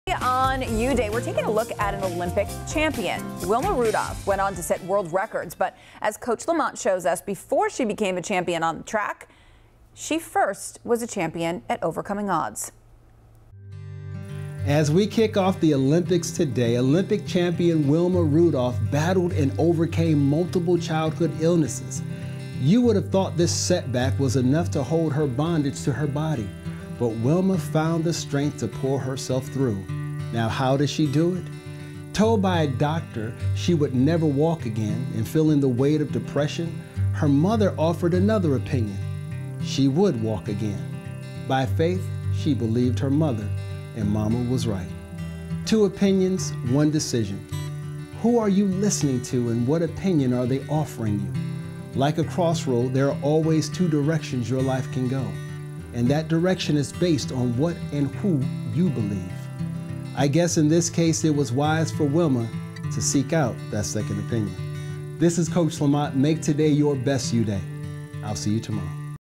YouDay: Wilma Rudolph
As we kick off the Olympics today, Olympic champion Wilma Rudolph battled and overcame multiple childhood illnesses.